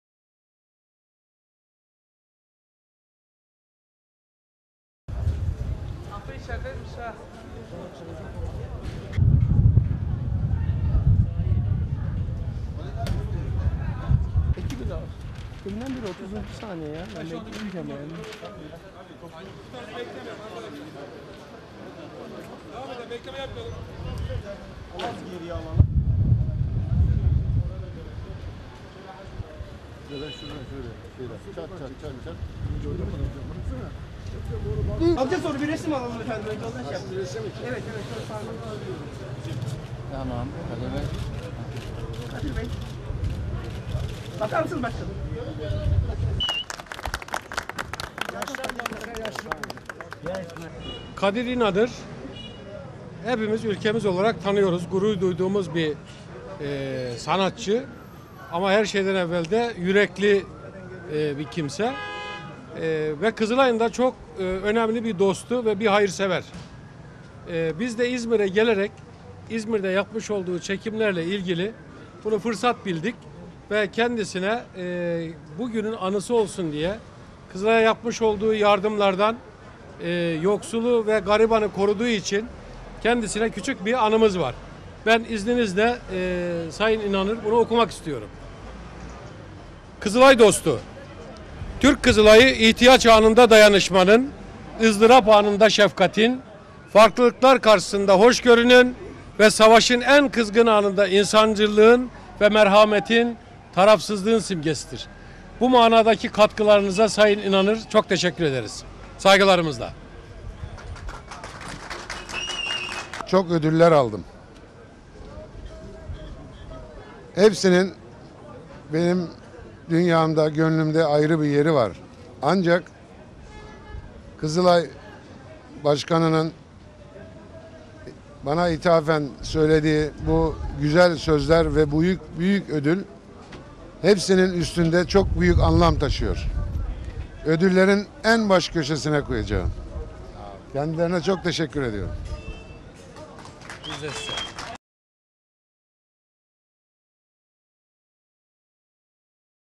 Türk Kızılayı Genel Başkanı Tekin Küçükali'den Sanatçı Kadir İnanır'a Teşekkür Plaketi. 04.02.2011
İZMİR - Türk Kızılayı Genel Başkanı Tekin Küçükali, derneğe katkılarından dolayı sanatçı Kadir İnanır'a teşekkür plaketi verdi.